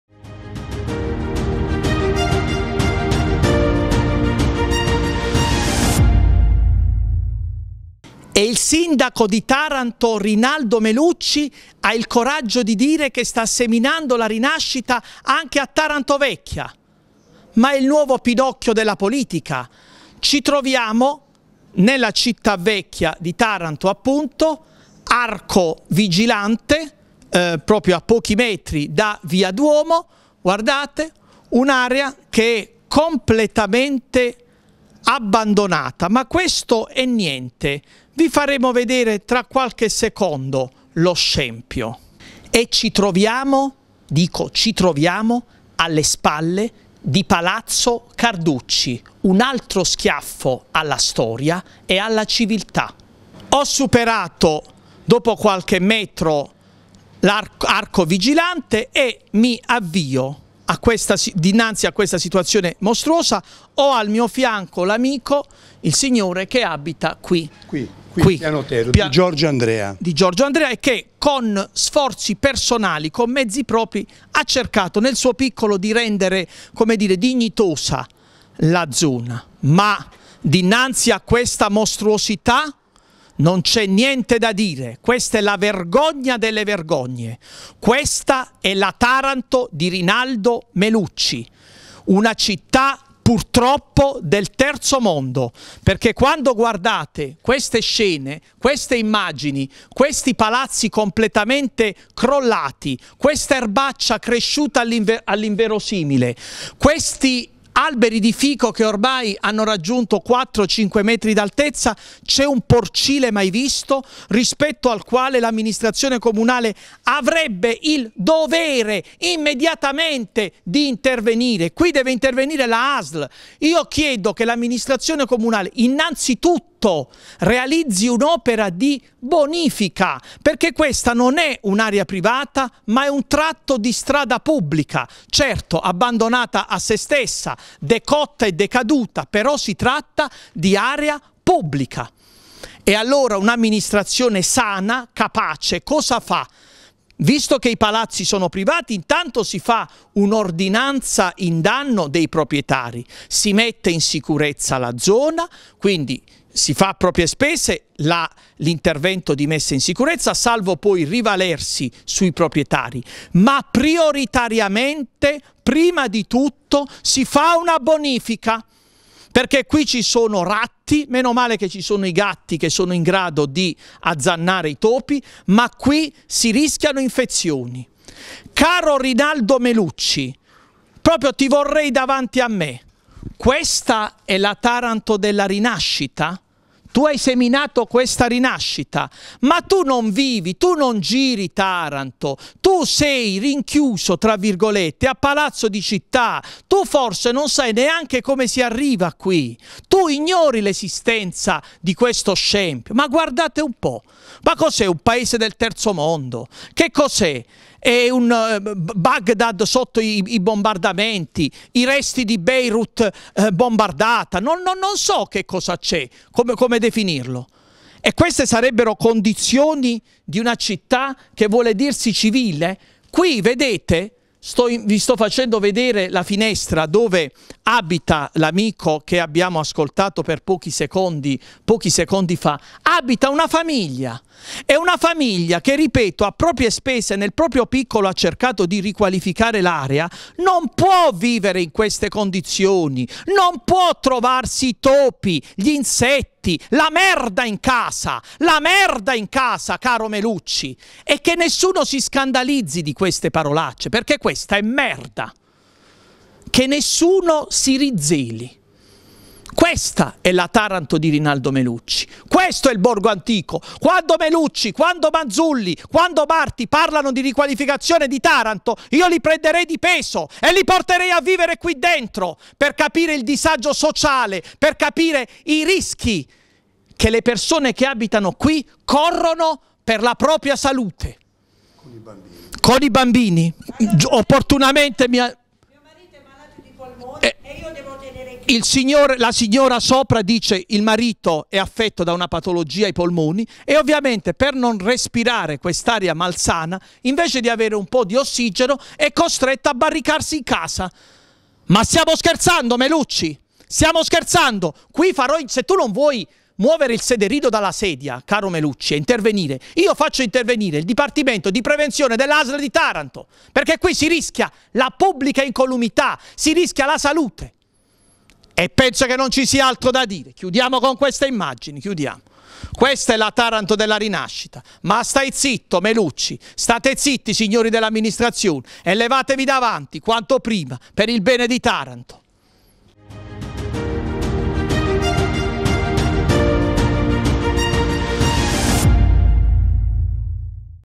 TARANTO - CITTÀ VECCHIA: ARCO VIGILANTE. DEGRADO IMPRESSIONANTE
TARANTO - CITTÀ VECCHIA, ARCO VIGILANTE (A POCHI METRI DA VIA DUOMO): DEGRADO, SPORCIZIA, VEGETAZIONE SPONTANEA E MACERIE IMPRESSIONANTI.
E questa sarebbe la rinascita che l'amministrazione Melucci dice di aver seminato?
E questa sarebbe la riqualificazione del Borgo Antico?
Queste purtroppo sono immagini di una città del terzo mondo quale Taranto, in mano a Melucci & company, è tristemente diventata.